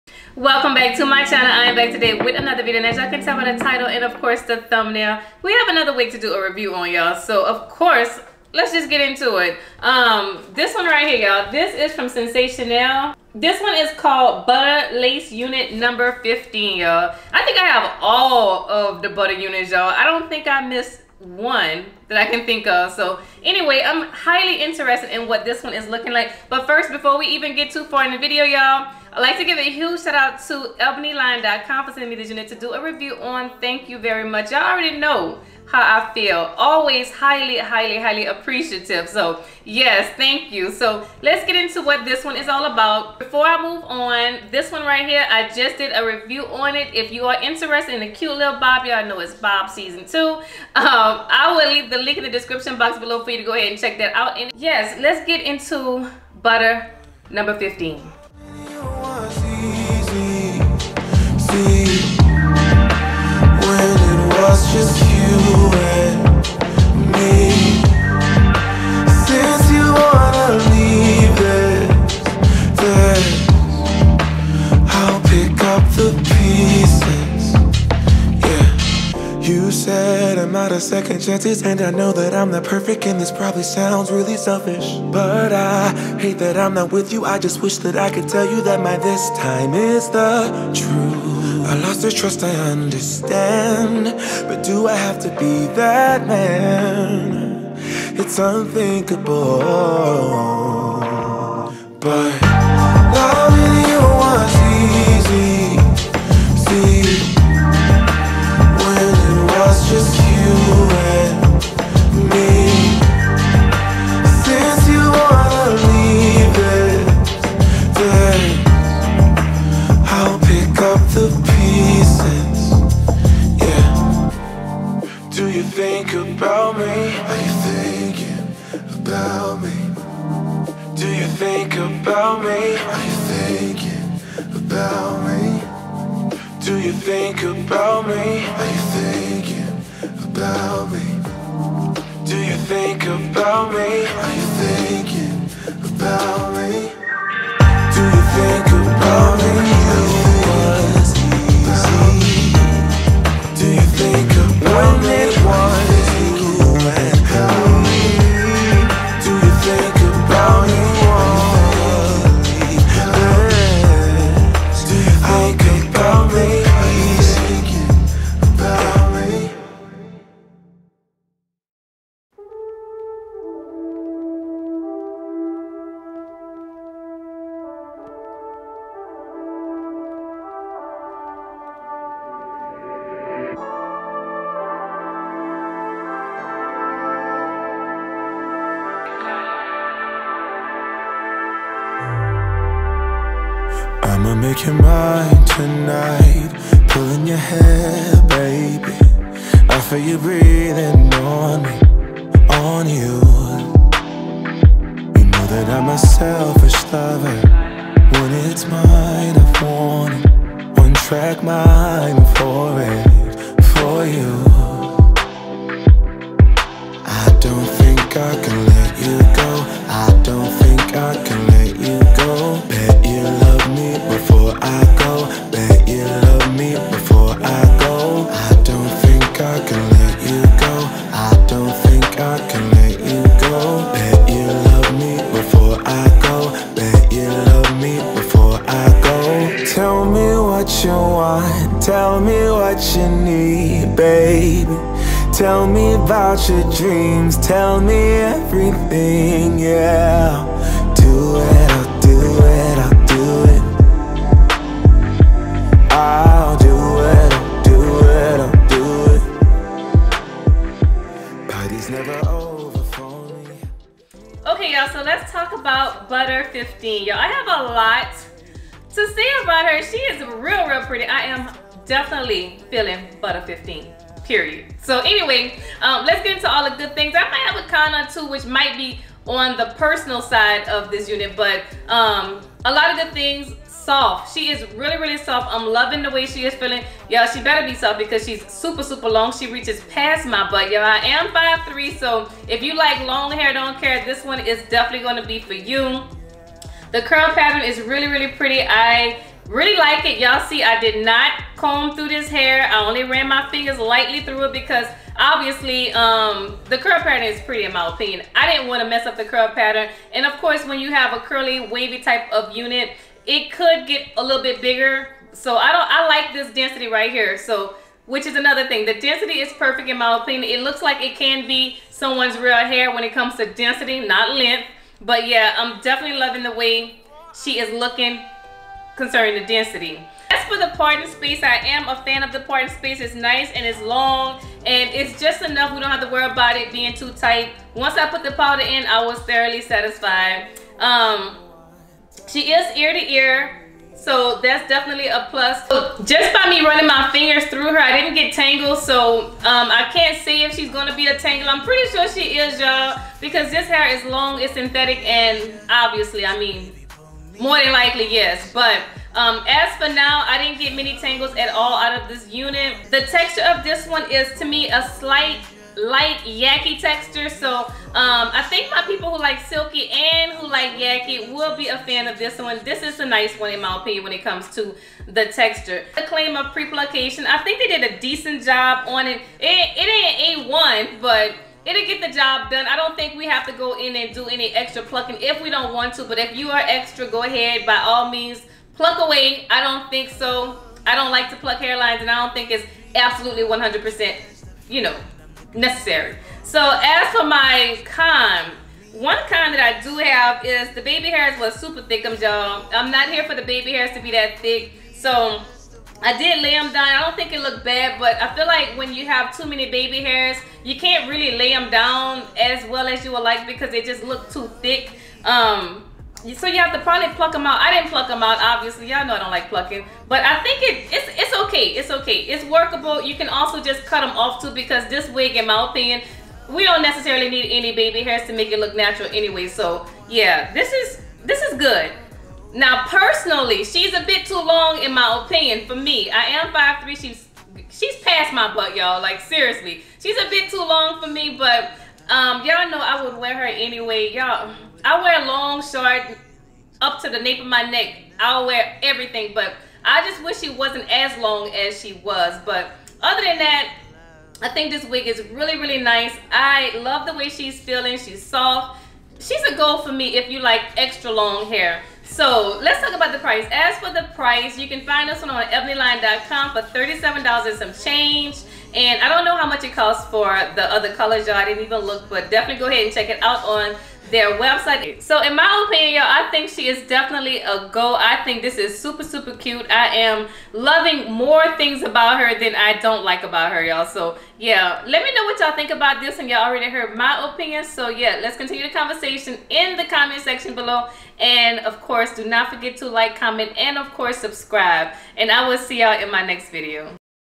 NEW SENSATIONNEL BUTTA LACE UNIT 15 FT. Ebonyline.com
Hi everyone here with another unit from Sensationnel Butta 15 wig so please check it out.

YanasEyes lashes: Yanaseyes.biz

all products